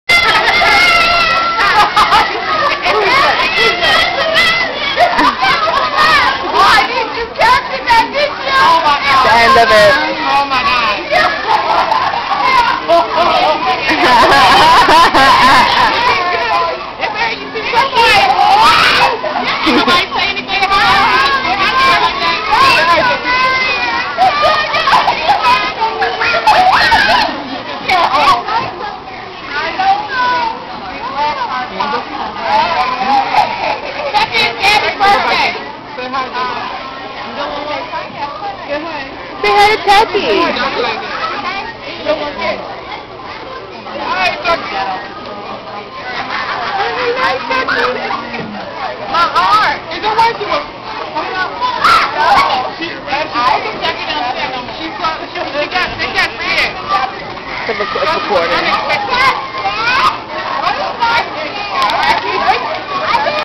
Gabrielle Victoria 2nd Birthday Celebration - Surprise Visit from Chuck E
Surprise Visit from Chuck E